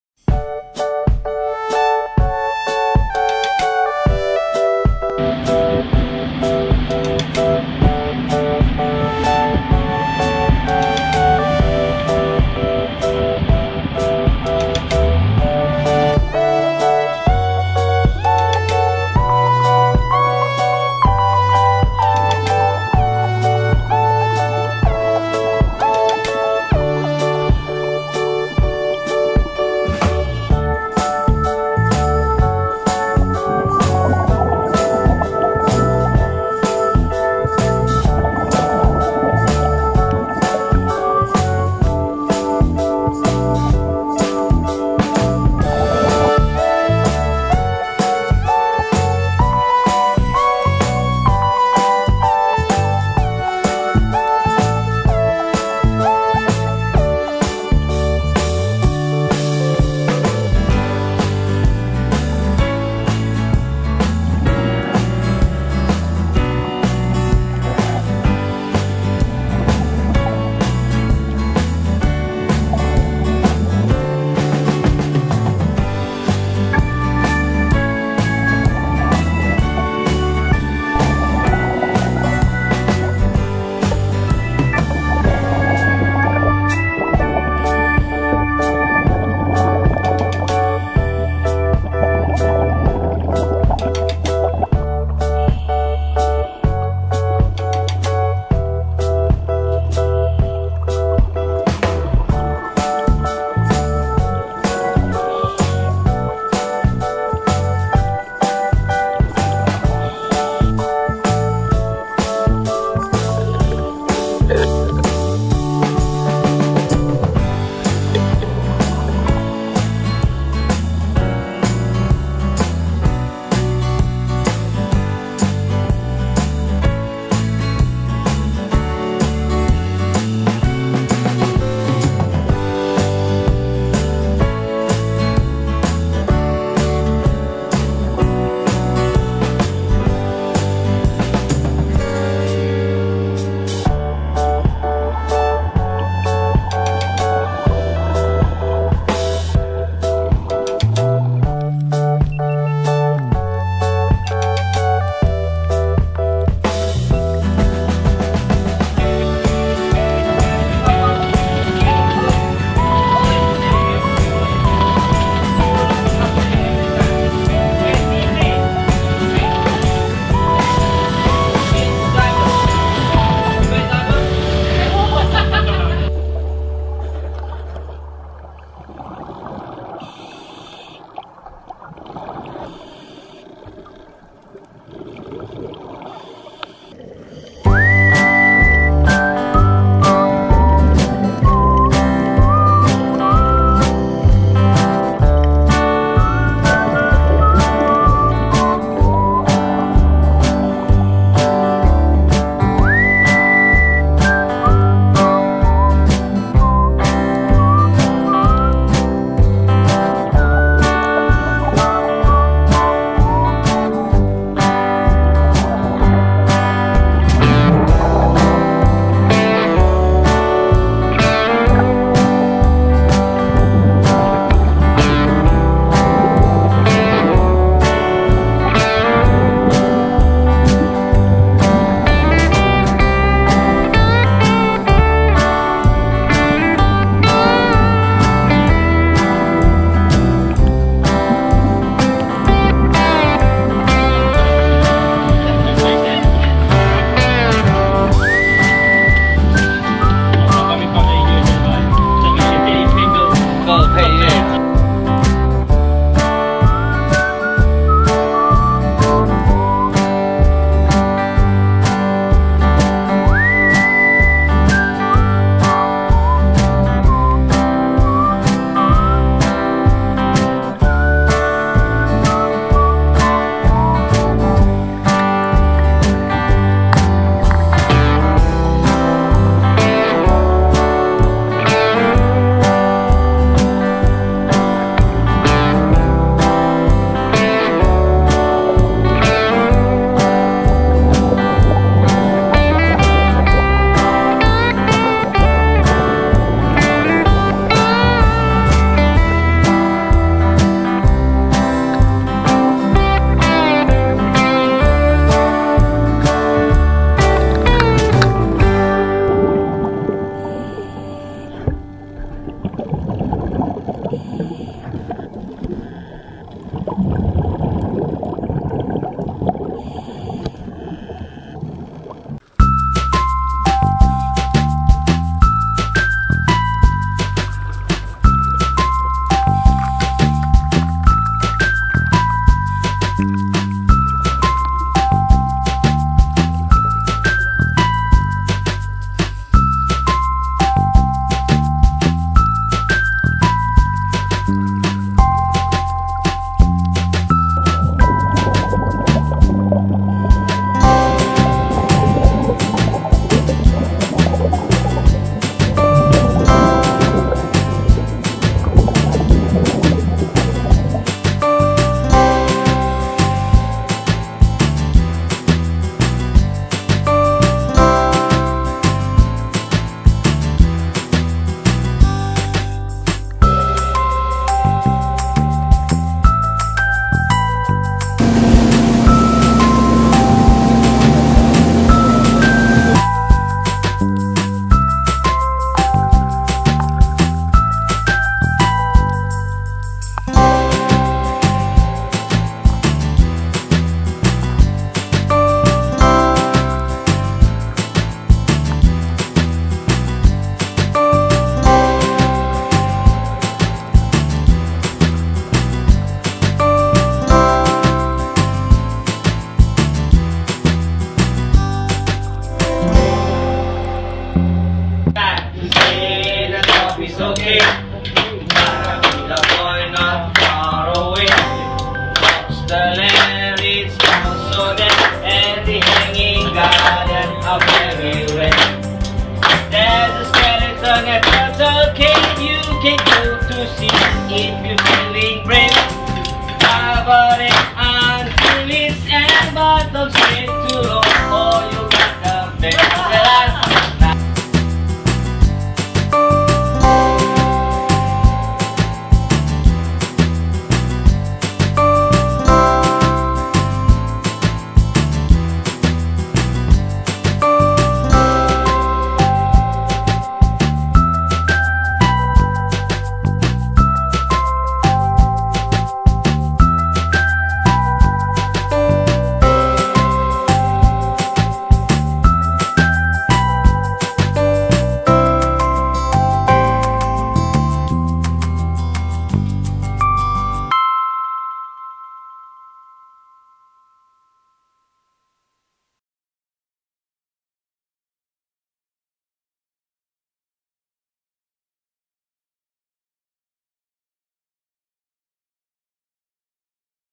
Sipadan-Mabul-Kapalai Diving 2015 full HD
Aug. 7 ~ Aug.12 2015, our Sipadan liveboard diving (MV Celebes Explore) video!